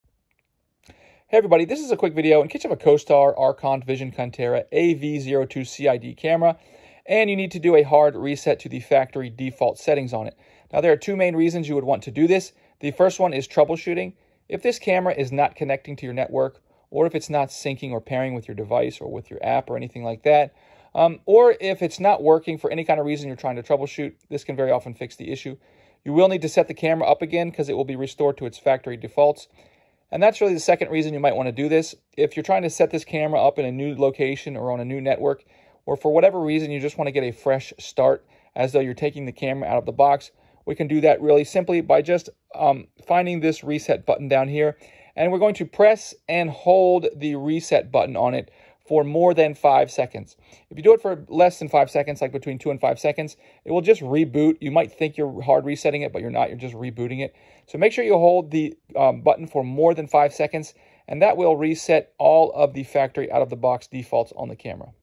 Hard Reset Arecont AV02CID Camera
How to troubleshoot or setup starting with a hard reset of a AV Costar Arecont Vision AV02CID-200 ConteraIP 2MP Indoor IR Camera to factory default settings if it is not working or not connecting or if it is frozen or not responding, the factory reset is a good troubleshooting step. You can also restore the factory settings to setup your device as new.  Found in the Arecont Vision AV02CID-200 ConteraIP 2MP Indoor IR Camera user manual.